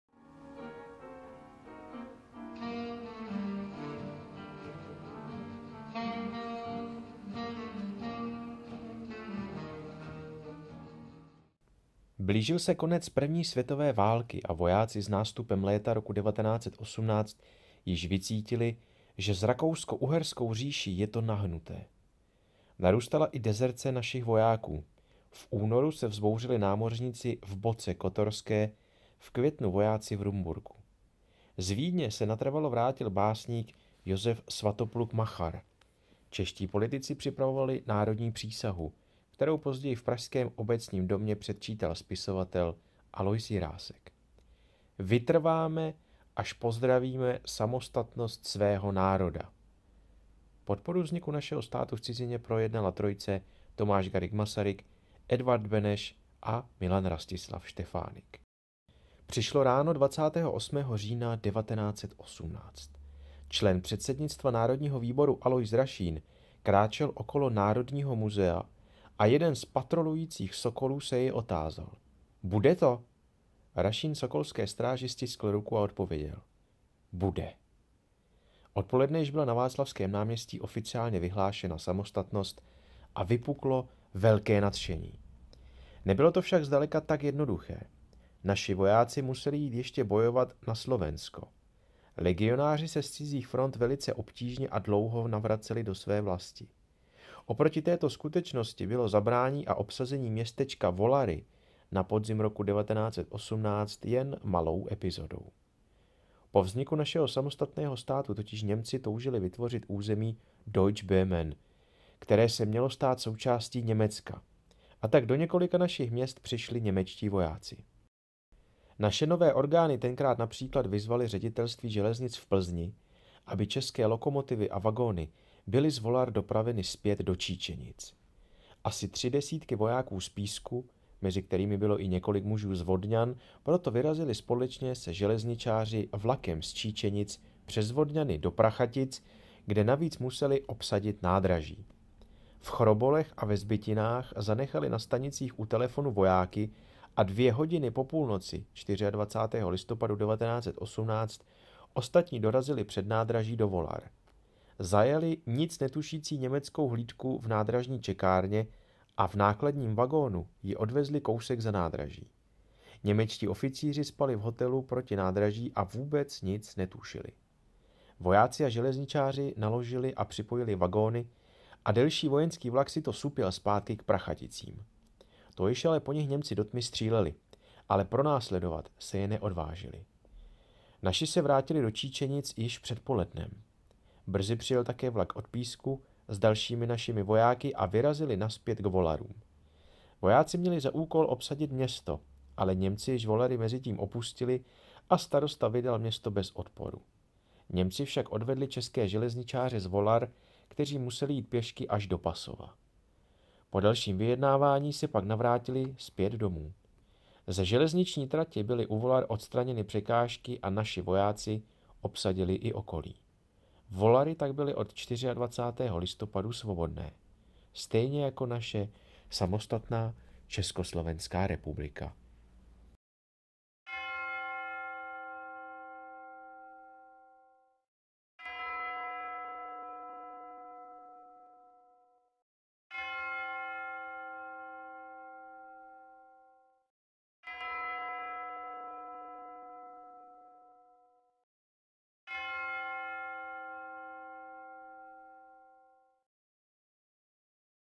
Volarská epizoda při vzniku ČSR (1918)
Při vzniku samostatného Československa v roce 1918 se na mnoha místech střílelo a bojovalo o území. Jedním ze známých případů je boj o vlakové nádraží ve Volarech.

Autor textu: František Dostál